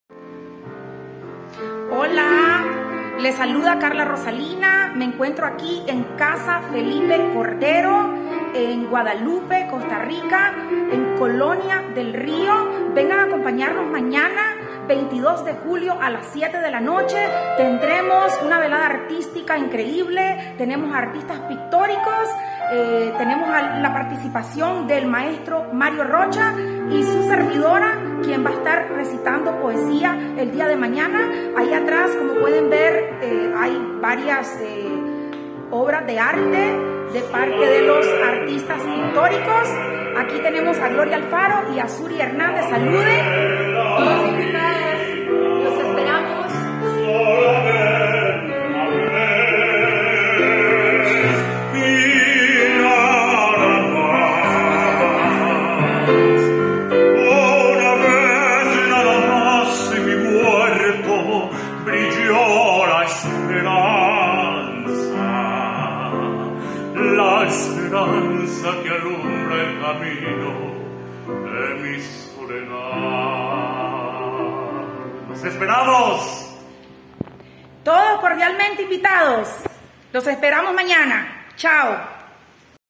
Mosaico Bohemio este sábado 22 de julio de 2023 en Casa Felipe Cordero.
Preparativos del concierto Mosaico Bohemio este sábado 22 de julio de 2023 en Casa Felipe Cordero, 7:00 pm colonia del río en Guadalupe San José. ¡¡Le esperamos!!